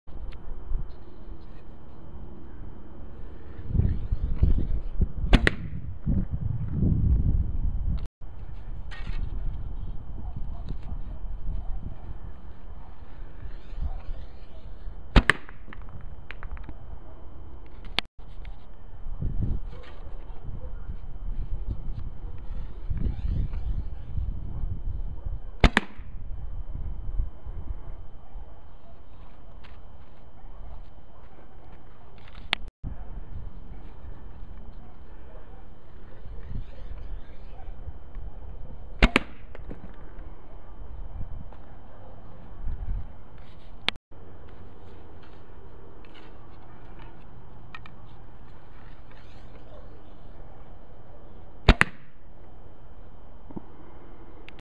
294 - First Five Test Shots with the Camera Just Below My Eyes
Positioning the camera and adjusting my shooting stance for these shots was a little tricky.  I basically had to shoot the bow with the camera in front of me, on a stationary stand, and just below my eyes.  It did get easier after several shots, though, and I think I can improve what the camera sees with a little more practice.

The last shot in this sequence is the one I used, and slowed down, in the previous video.

Created with youtube editor.